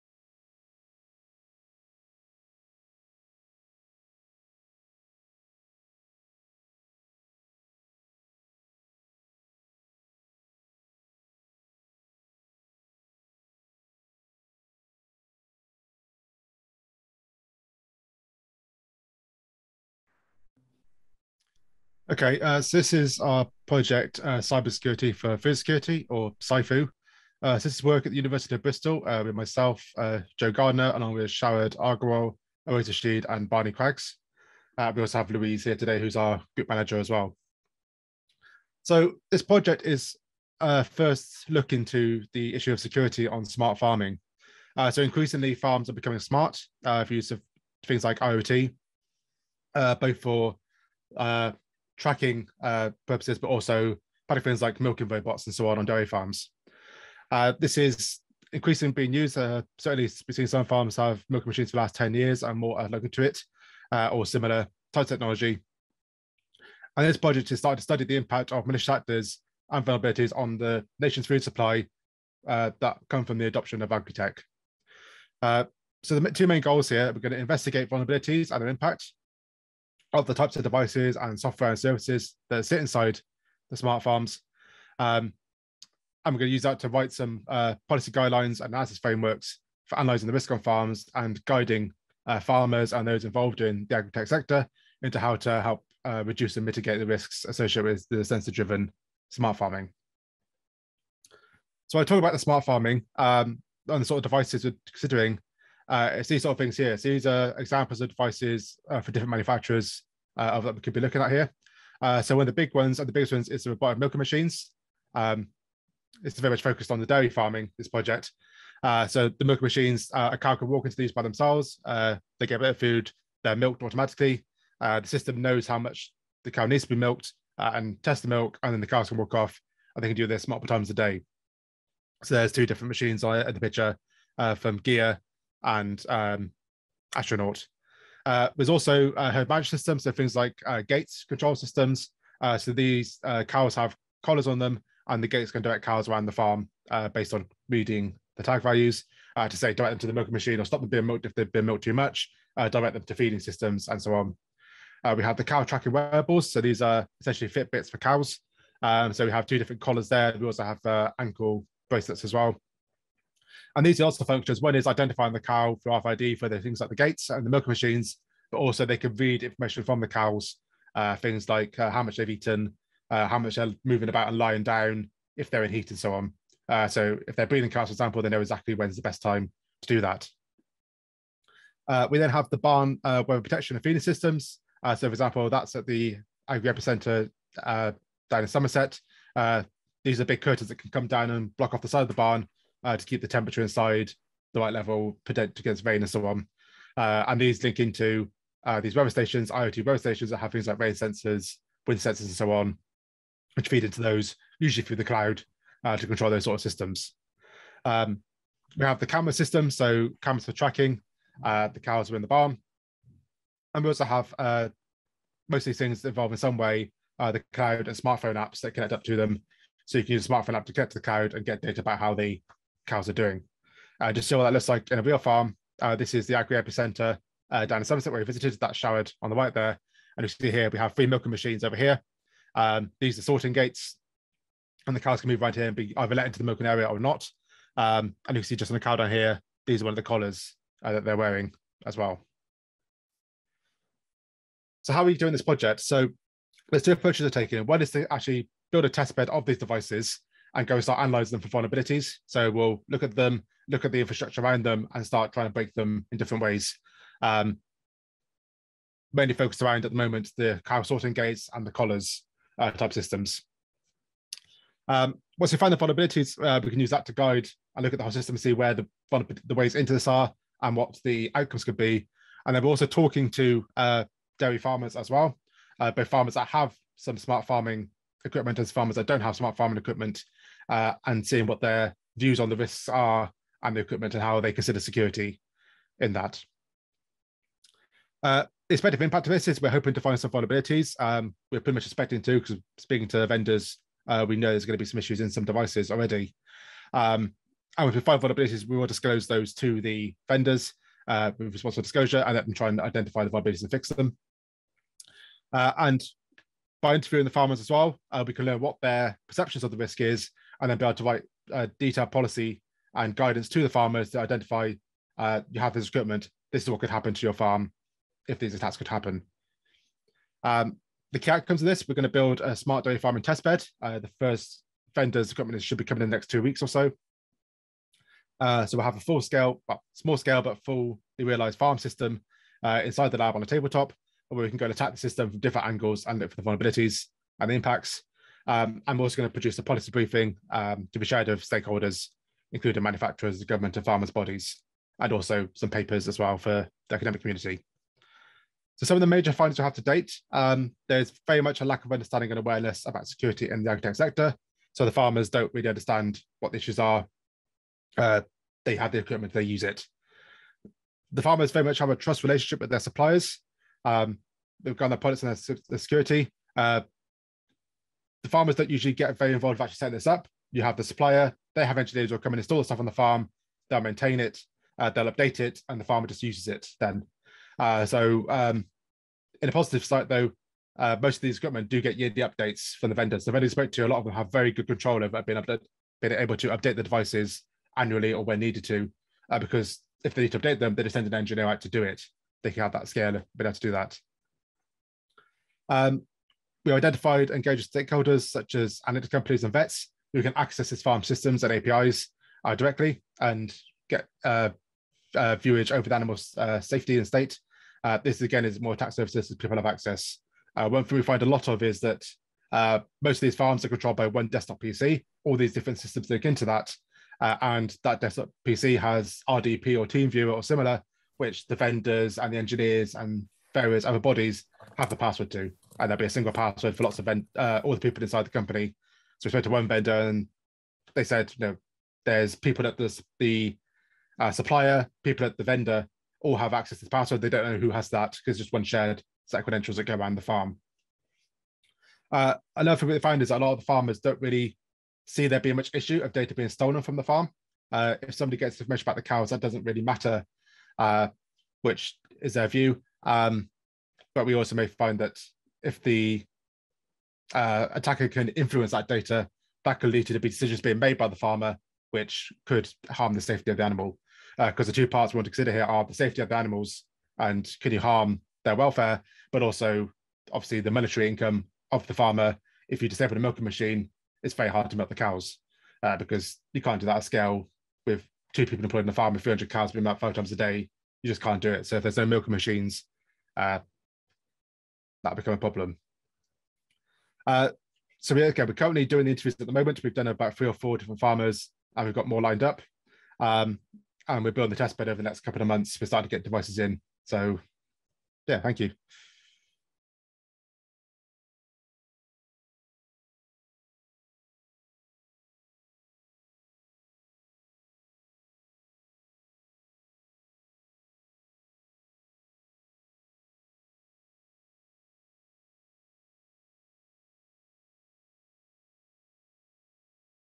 PETRAS Securing the Future, November 2021: CyFoo Poster Presentation
During 2021, the PETRAS National Centre of Excellence for IoT Systems Cybersecurity ran two public events as part of the SDTaP programme: the PETRAS Securing the Future event series in June and November 2021. Each event created a space for over 100 attendees from the research community and new and existing partners to discuss pressing topics relating to securing edge technologies.

Highlights from November’s event included knowledge exchange between academia, industry and government, panel discussions relating to cybersecurity of IoT for industry and the challenges of agile resilience in urban environments. In addition, the PETRAS Securing the Future event in November provided an online space for poster presentations by PETRAS research teams and their research ‘User Partners’.